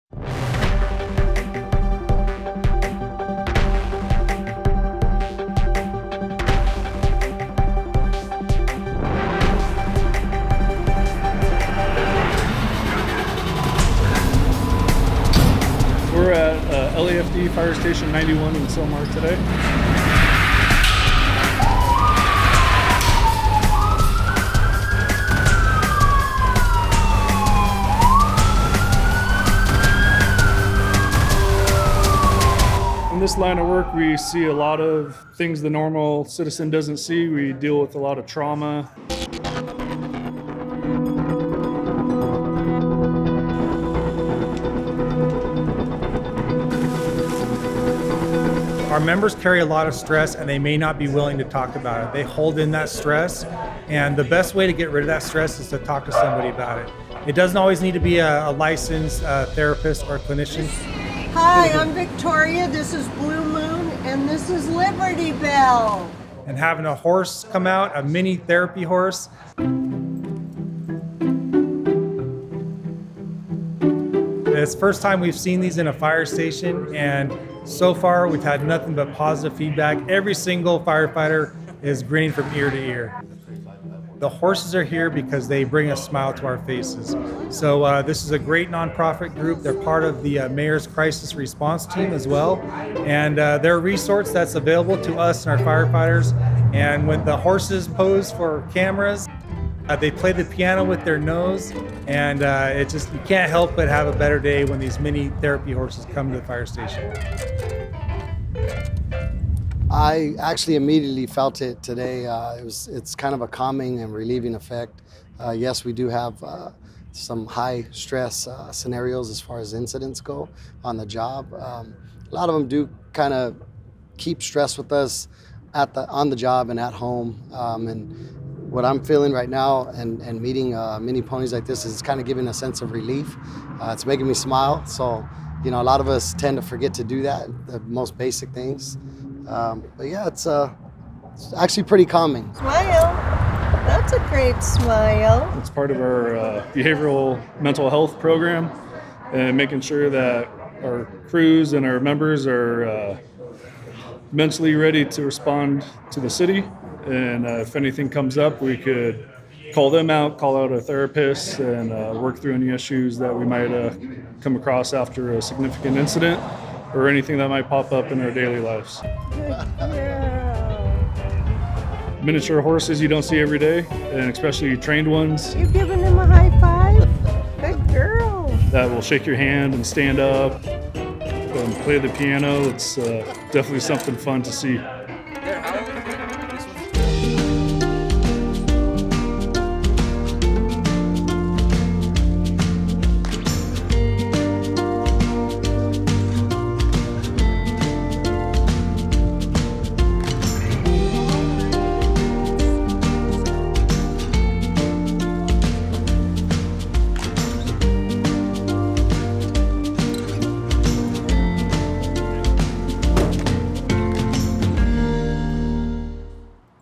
LAFD: Mini Therapy Horses Visit Fire Station 91
Mini therapy horses from the Mayor's Crisis Response Team recently paid a visit to Fire Station 91 in Sylmar.

Los Angeles Fire Department firefighters and paramedics regularly deal with traumatic, life threatening - and sometimes disturbing situations.
These special horses are among the innovative resources L.A.'s first responders can access to relieve post traumatic occupational stress and support personal and team wellness.